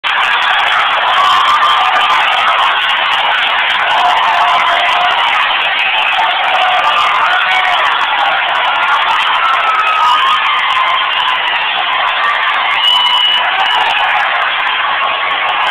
Pandemonium at the Shoe as UM falls to Appalachian State
Taken a few moments after the end of the Michigan Appalachian State game as seen from Ohio Stadium